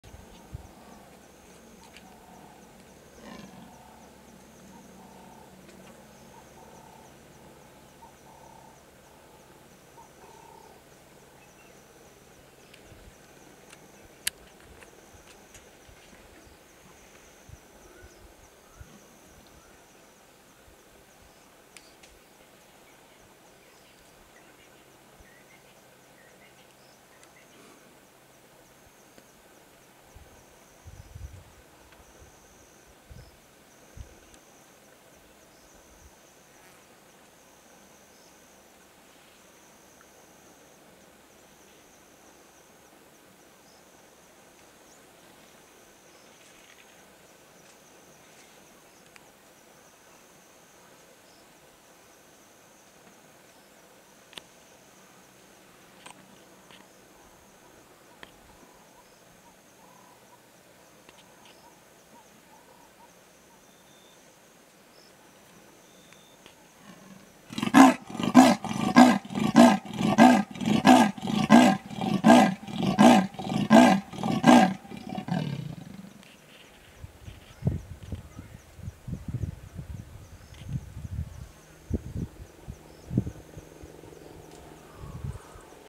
Leopard Rasping
Female Leopard grooming, yawning and then an amazing territorial rasp whilst looking at the camera

filmed on Toulon, Sabi Sands, greater Kruger with a 7dmk2 and a mk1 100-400 lens